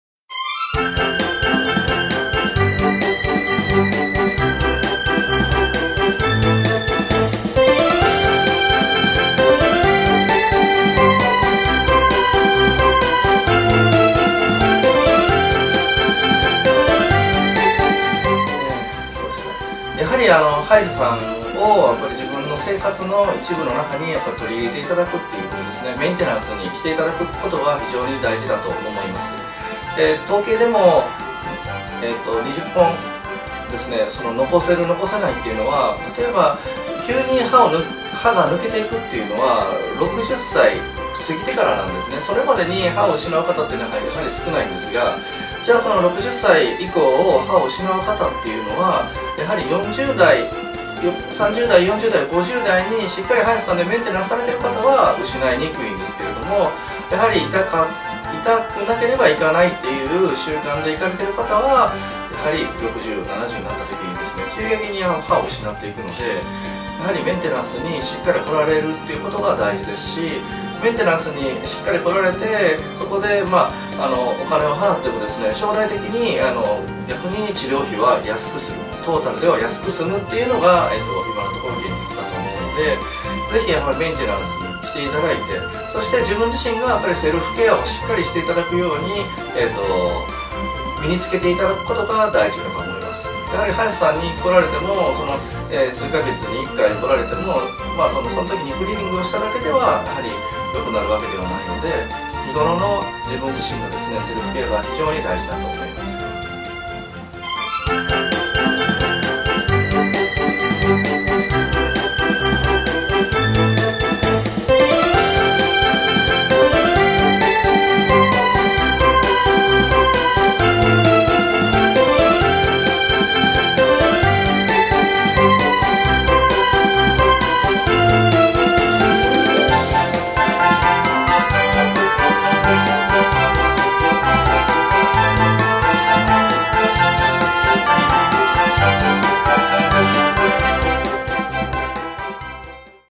歯科医 大阪｜南森町の歯科医院・いちき歯科の8020運動のメンテナンス重要性。
健康で丈夫な歯を残すための方法や注意事項について説明しています。いつまでも元気で­健康な歯をキープして食事も楽しみましょう！メンテナンスも定期的に受けましょう。い­ちき歯科の院長、市来正博です。弊社のホームページ：